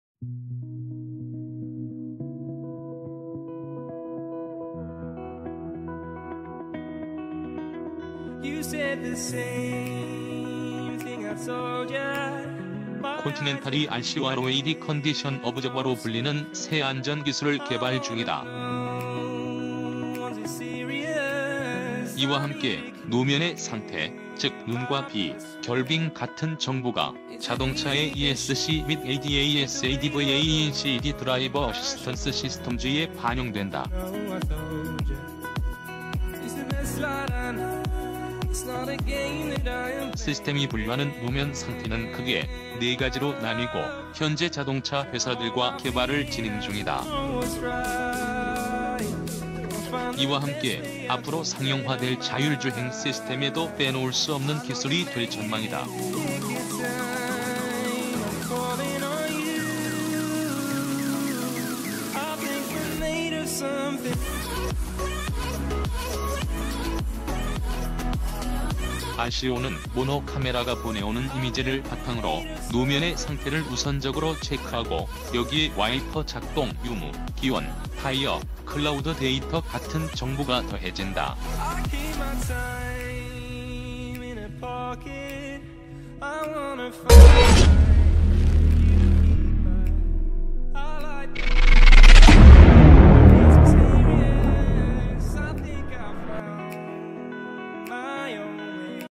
콘티넨탈의 첨단 로드 컨디션 옵저버, 노면을 읽는 관찰자
,자율주행,콘티넨탈,콘티넨탈 Road Condition Observer,콘티넨탈 로드 컨디션 옵서버,콘티넨탈 로드 컨디션 옵저버,콘티넨탈 타이어
콘티넨탈의 첨단 로드 컨디션 옵저버, 노면을 읽는 관찰자 AppStore 콘티넨탈이 RCORoad Condition Observer로 불리는 새 안전 기술을 개발 중이다. RCO는 말 그대로 노면을 읽는 기술이다. 차에 달린 센서와 클라우드 데이터를 연동해 노면의 정보를 실시간으로 파악한다. 이를 통해 교통사고를 최소화 할 수 있다는 설명이다. 콘티넨탈에 따르면 차의 센서와 클라우드 데이터는 실시간으로 통신한다. 이와 함께 노면의 상태, 즉 눈과 비, 결빙 같은 정보가 자동차의 ESC 및 ADASAdvanced Driver Assistance Systems에 반영된다. 시스템이 분류하는 노면 상태는 크게 4가지로 나뉘고, 현재 자동차 회사들과 개발을 진행 중이다. 현재는 운전자가 노면의 상태를 직접 파악한다. 하지만 RCO가 상용화 되면 운전자가 시각적으로 판단하는 것보다 훨씬 정확하게 노면의 상태를 읽을 수 있다. 이와 함께 앞으로 상용화될 자율주행 시스템에도 빼놓을 수 없는 기술이 될 전망이다. RCO는 모노 카메라가 보내오는 이미지를 바탕으로 노면의 상태를 우선적으로 체크하고, 여기에 와이퍼 작동 유무, 기온, 타이어, 클라우드 데이터 같은 정보가 더해진다. 만약 노면이 미끄럽다고 판단될 경우 ESC의 작동 모드를 그에 맞게 변경하기 때문에 일반적인 운전자보다 안전하다는 설명이다.